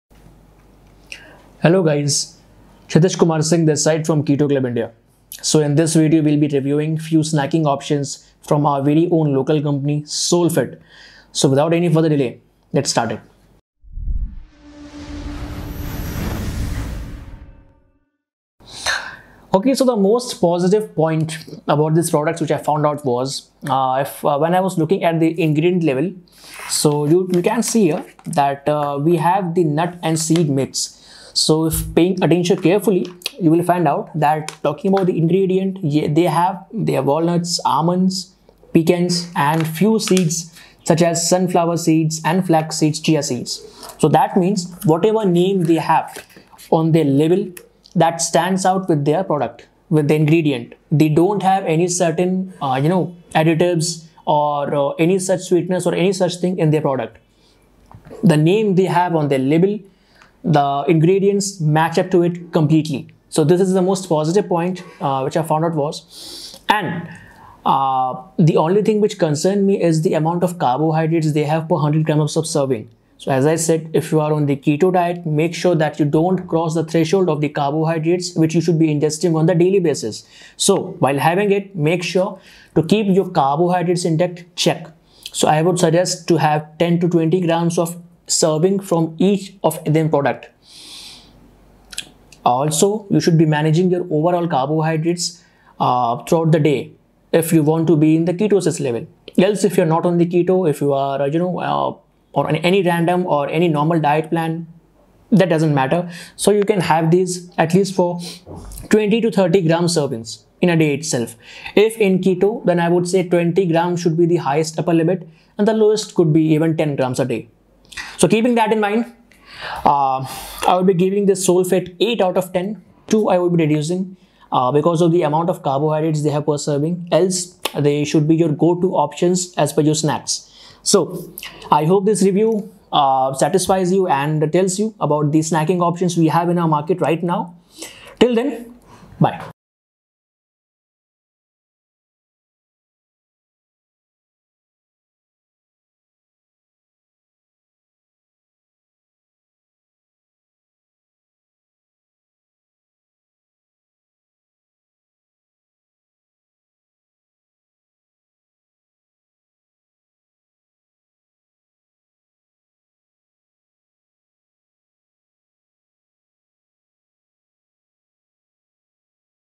Keto Diet Snacks Options In India by Soulfit
So in this video, we are reviewing the healthy snack options while one is following a Keto Diet Plan. Even if not on any such diet protocols, u can consume them without any hesitation as they are much healthier and could provide you with some vitamins & minerals if not much.

Nutberry Mix, Nut & Seeds mix, Seed Mix, and Nut mix from Soulfit would come really handy as your snacking option as it will suffice you and could also help in cutting down the carbs craving.